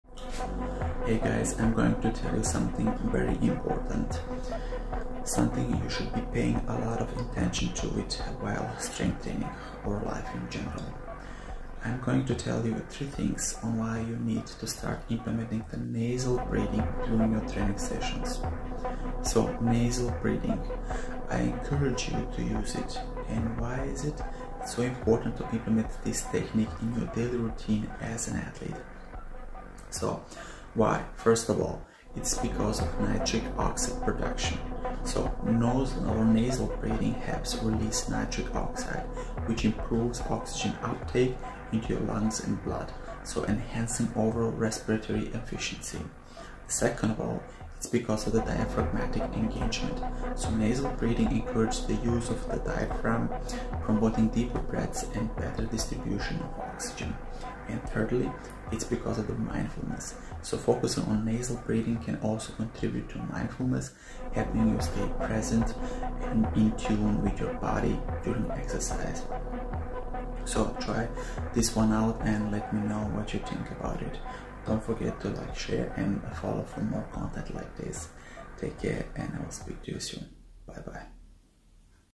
‼️ START USING THIS BREATHING TECHNIQUE ‼️
1 Nitric Oxide Production:Nose [ or nasalbreathing ]breathing helps release nitric oxide, which improves oxygen uptake in your lungs and blood, enhancing overall respiratory efficiency.

2. Diaphragmatic Engagement:Nasal breathing encourages the use of the diaphragm, promoting deeper breaths and better distribution of oxygen.

3. Mindfulness:Focusing on nasal breathing can also contribute to mindfulness, helping you stay present and in tune with your body during exercise.

Here's a tool that it can help you improve nasal breathing skills:

It could be worn during training sessions.